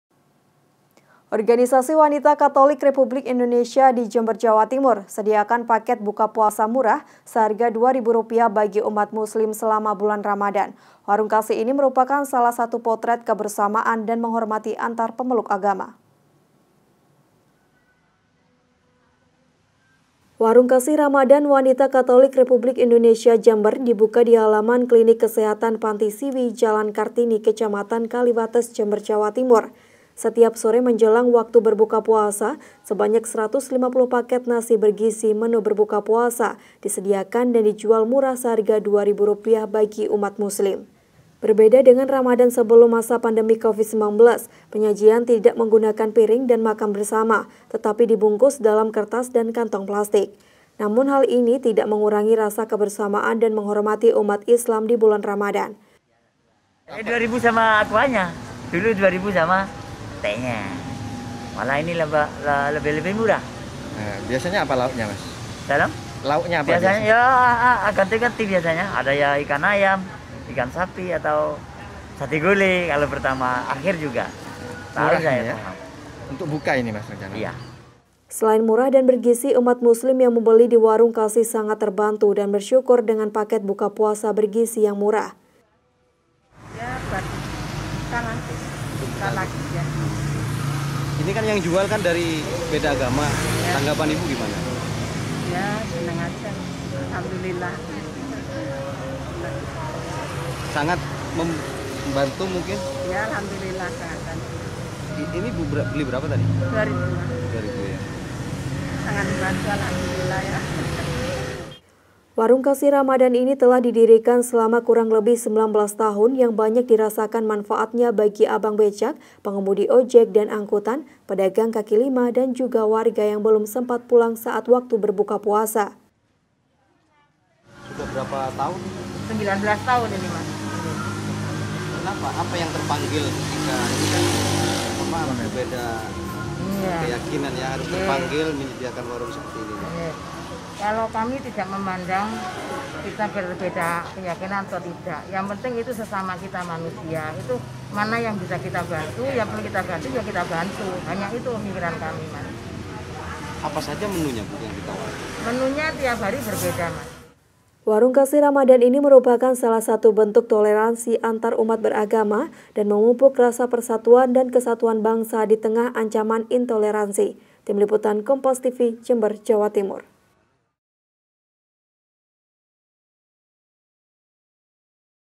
Organisasi Katolik Buka Warung Murah Untuk Berbuka Puasa
JEMBER, KOMPAS.TV - Organisasi Wanita Katolik Republik Indonesia, di Jember, Jawa Timur, sediakan paket buka puasa murah seharga dua ribu rupiah bagi umat muslim selama bulan ramadan. Warung kasih ini merupakan salah satu protret kebersamaan dan menghormati antar pemeluk agama.

Warung kasih ramadan Wanita Katolik Republik Indonesi Jember, dibuka di halaman klinik kesehatan Panti Siwi, Jalan Kartini, Kecamatan Kaliwates, Jember, Jawa Timur.

Setiap sore menjelang waktu berbuka puasa sebanyak 150 paket nasi bergizi menu berbuka puasa disediakan dan dijual murah seharga dua ribu rupiah bagi umat muslim.

Berbeda dengan ramadan sebelum masa pandemi covid 19, penyajian tidak menggunakan piring dan makan bersama, tetapi dibungkus dalam kertas dan kantong plastik.

Namun hal ini tidak mengurangi rasa kebersamaan dan menghormati umat islam di bulan ramadan.

Selain murah dan bergizi, umat muslim yang membeli di warung kasih sangat terbantu dan bersyukur dengan paket buka puasa bergizi yang murah.

Warung kasih ramadan ini telah didirikan selama kurang lebih 19 tahun, yang banyak dirasakan manfaatnya bagi abang becak, pengemudi ojek dan angkutan, pedagang kaki lima dan juga warga yang belum sempat pulan saat waktu berbuka puasa.

Warung kasih ramadan ini merupakan salah satu bentuk toleransi antar umat beragama dan memupuk rasa persatun dan kesatuan bangsa ditengah ancaman intoleransi.